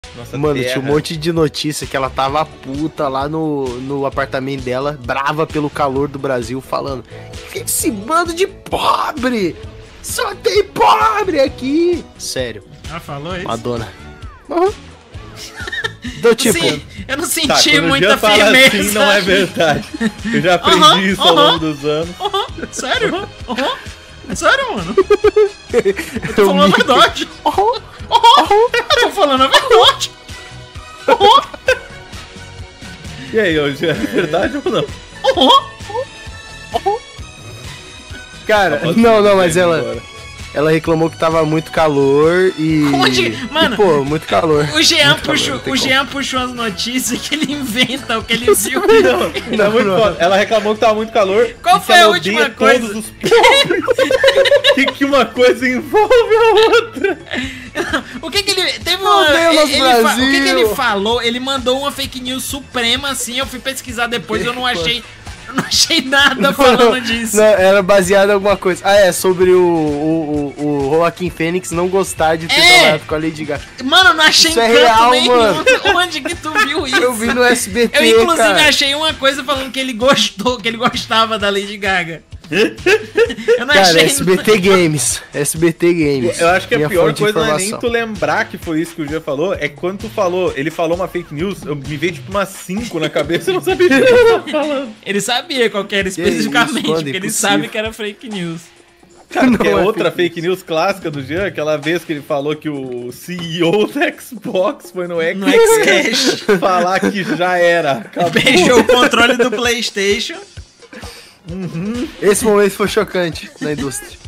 Jean e as suas True News
Se existe uma pessoa no Brasil que está verdadeiramente comprometida com a verdade e em transmitir os fatos, essa pessoa se chama Jean Dias de Paula! Um garoto simples do interior de São Paulo, desde o início de sua vida teve como projeto transmitir informações verídicas para todos que estivessem ao seu lado. Acompanhe os detalhes no vídeo.